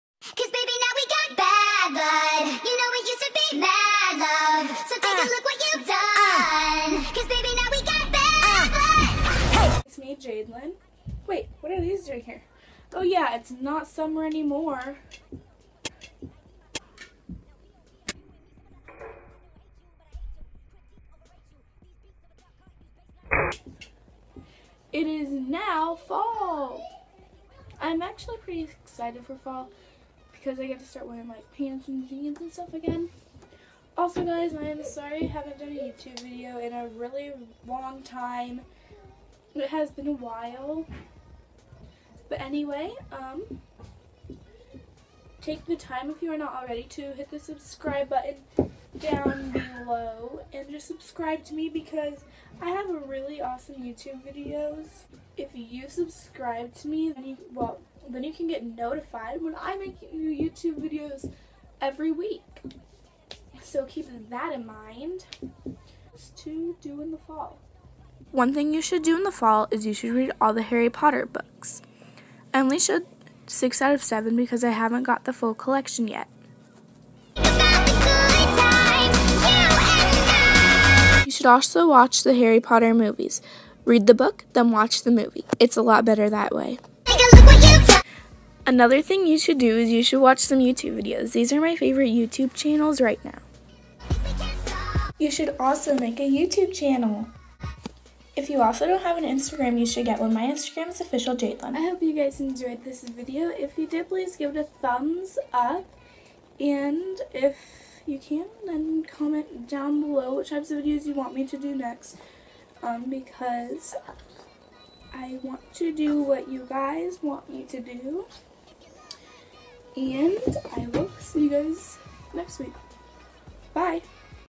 Things To Do In The Fall!!!🍁
Hey guys it's Jadelynn and today I decided to do what to do in the fall! I hope you enjoyed!!!